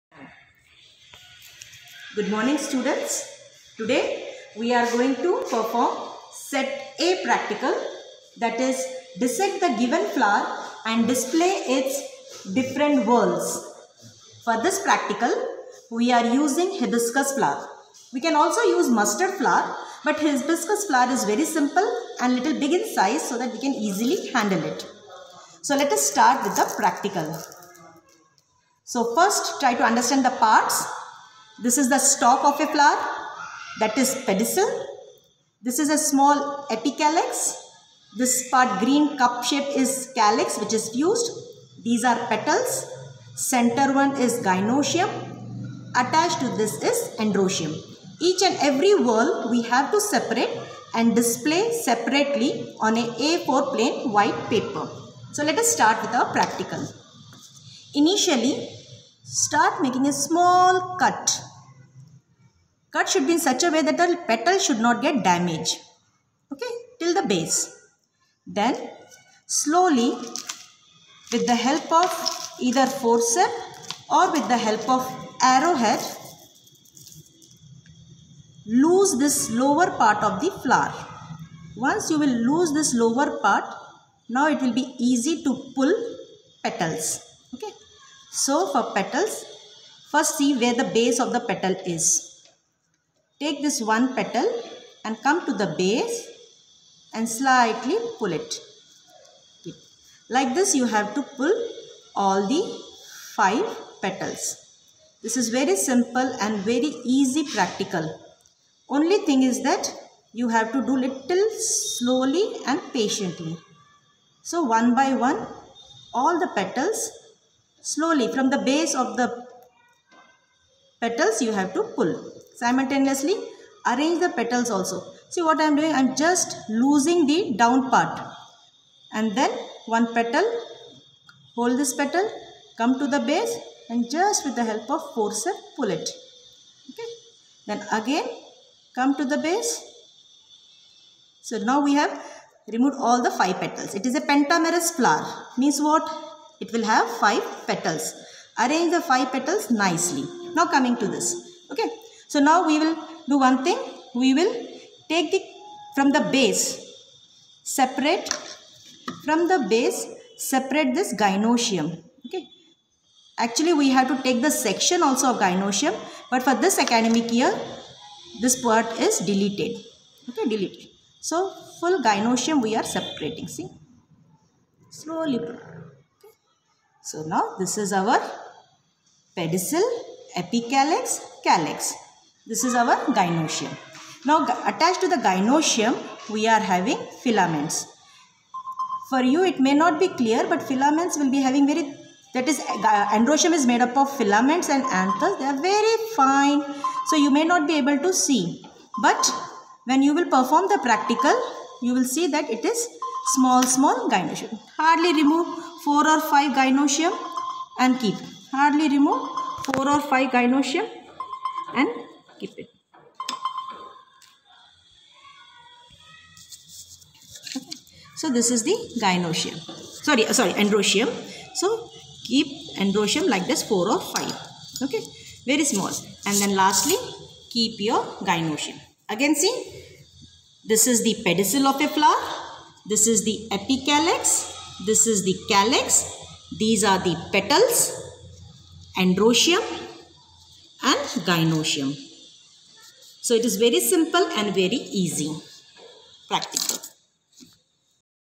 Dissect the given flower and display its different whorls
Set A Practical.
Q.1 Dissect the given flower (Hibiscus) and display its different whorls